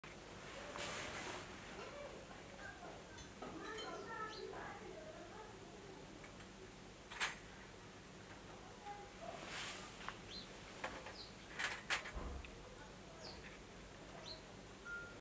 Ostriches synchronized drinking
two ostriches drinking from a water hole in Damaraland, Namibia